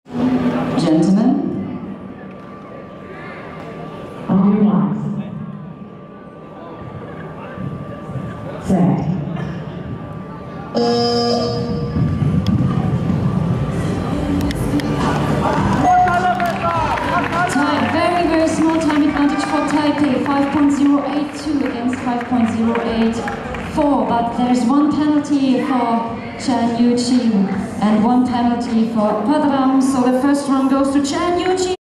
WFSC 2015 / Junior / SF1 men run 1
WORLD FREESTYLE SKATING CHAMPIONSHIPS 2015
TORINO ITALI 26-30 OCTOBER /
Junior / SF1 speed slalom men run 1